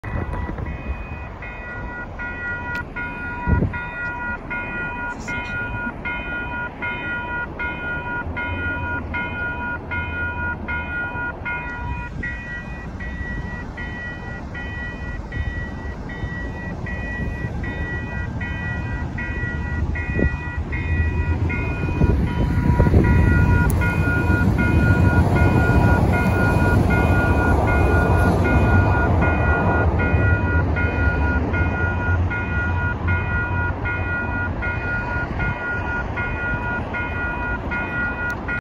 S200 train at James mckevitt road SW
Saw this c-train leaving fish creek lacombe station on the way to Starbucks